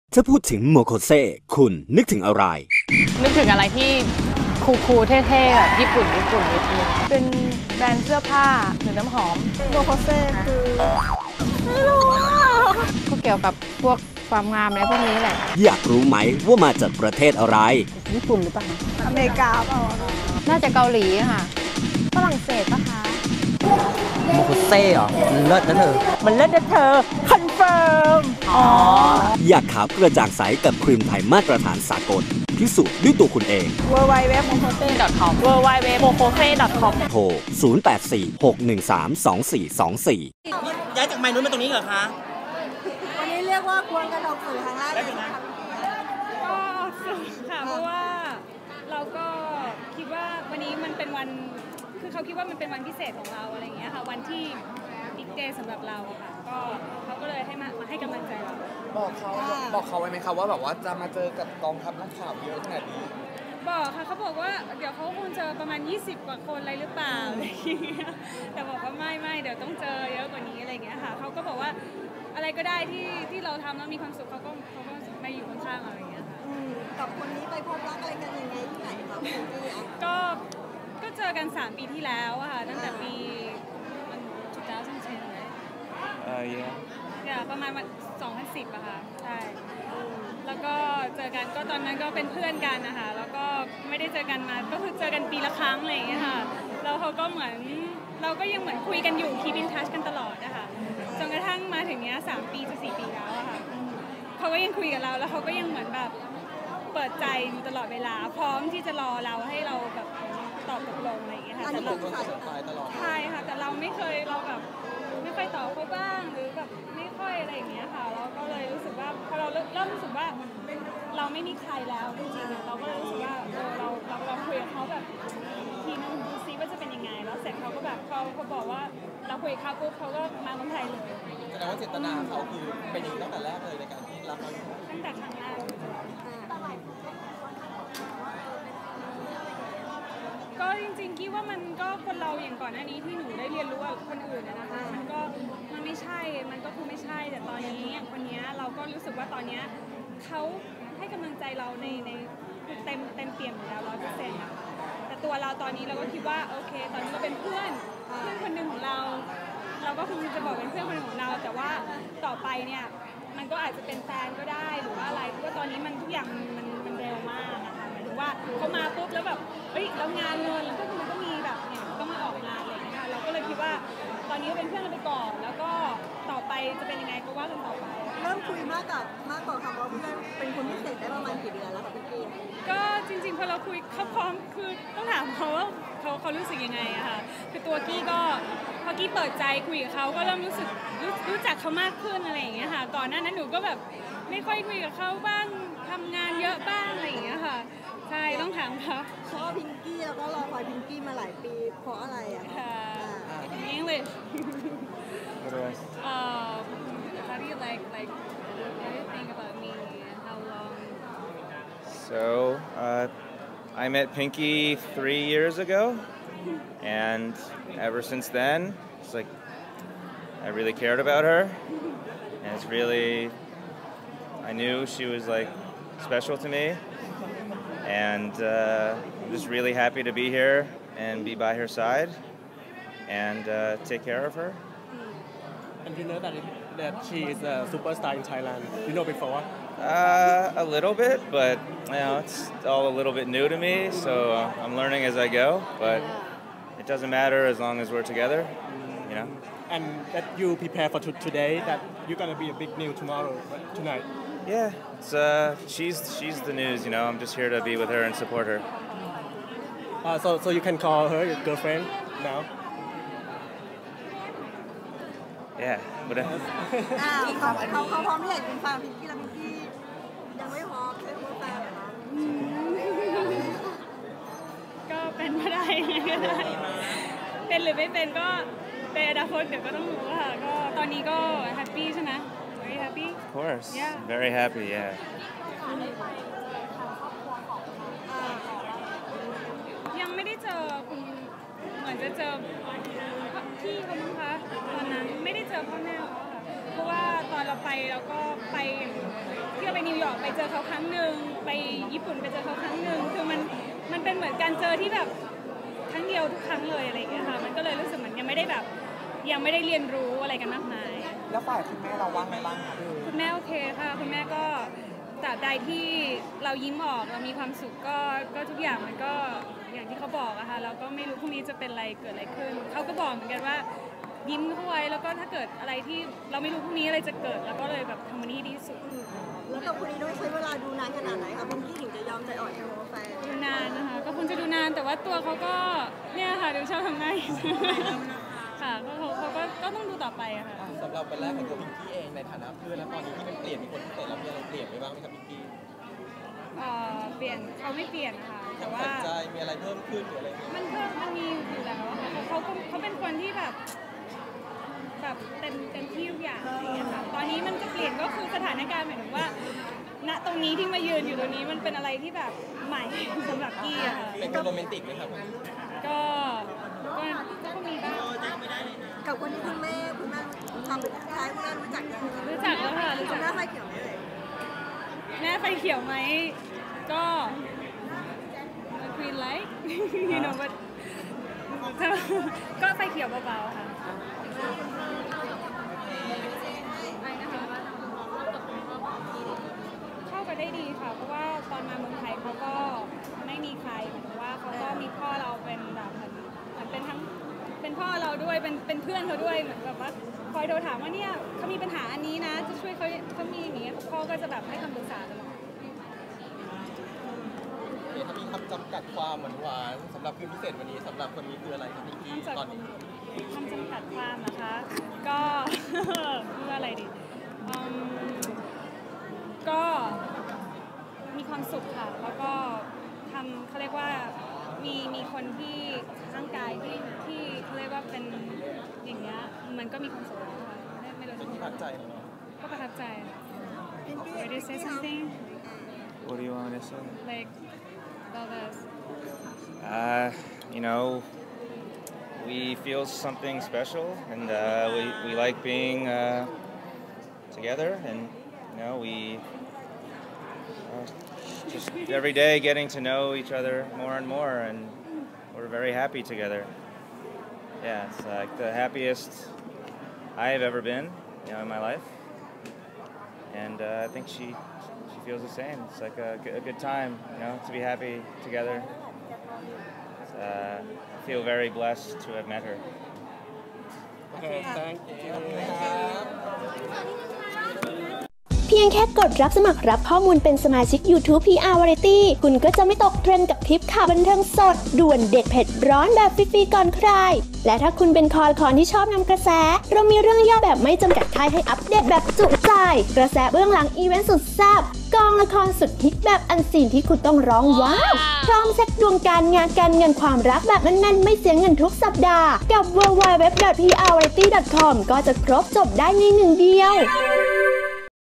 พิ้งกี้ โชว์หวานควง พอล หนุ่มนักธุรกิจลูกครึ่งคนสนิท ออกสื่อ
พิ้งกี้ โชว์หวานควง พอล หนุ่มนักธุรกิจลูกครึ่งคนสนิท ออกสื่อ ดูรูปและข่าวเพิ่มเติม